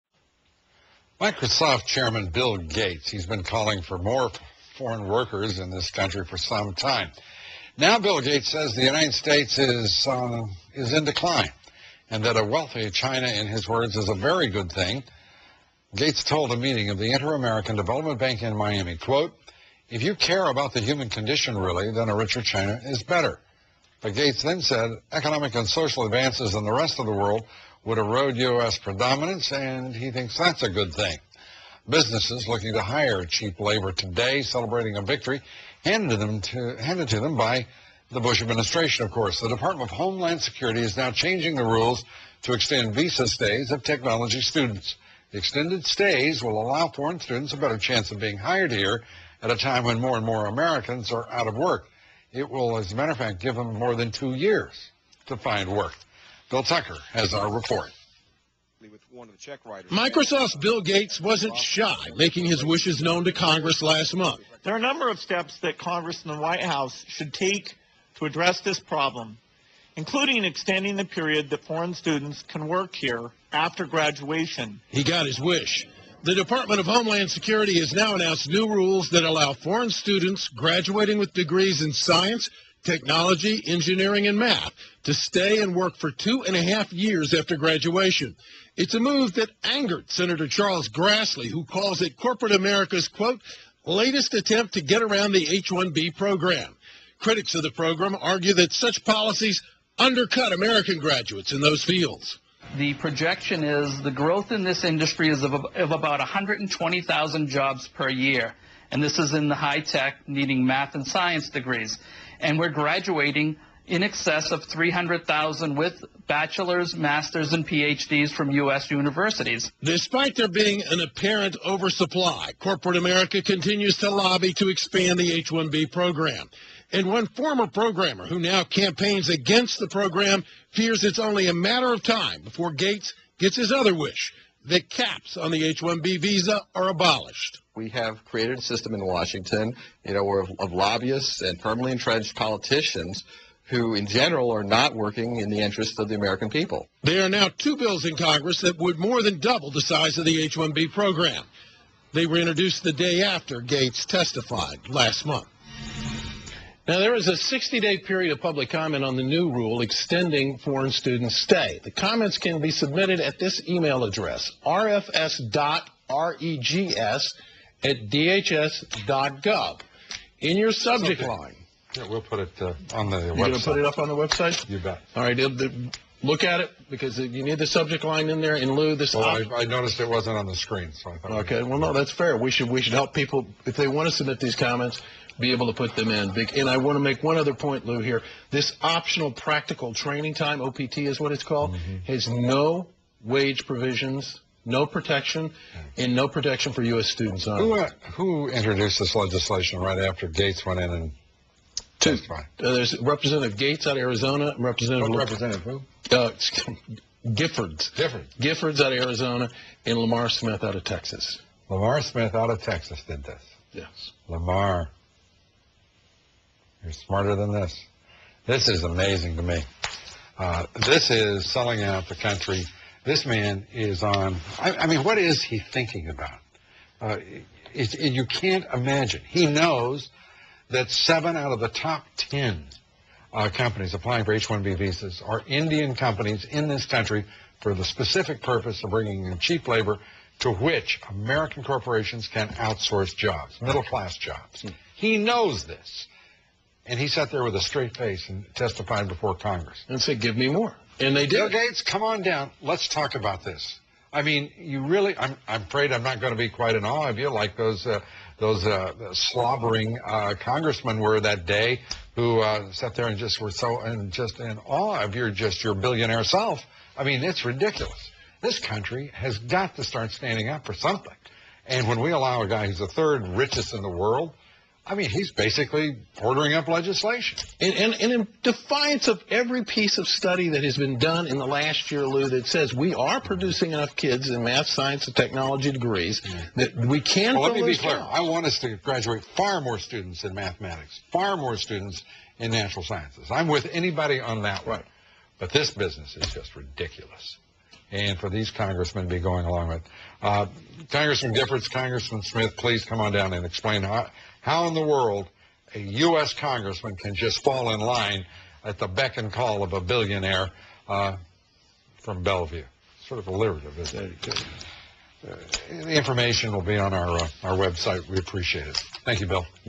DHS, Bush, bypass Congress, increase foreign guest workers
Lou Dobbs report on DHS, Bush, Chertoff,  per lobbyists demands, bypasses Congressional oversight and extends the F-1 OPT student Visa to work in the United States from 1 year to 2 years, 5 months.  Bill Gates wants to flood the STEM labor supply and shut out US citizen students from career starting opportunities in Science, Technology, Engineering and Mathematics.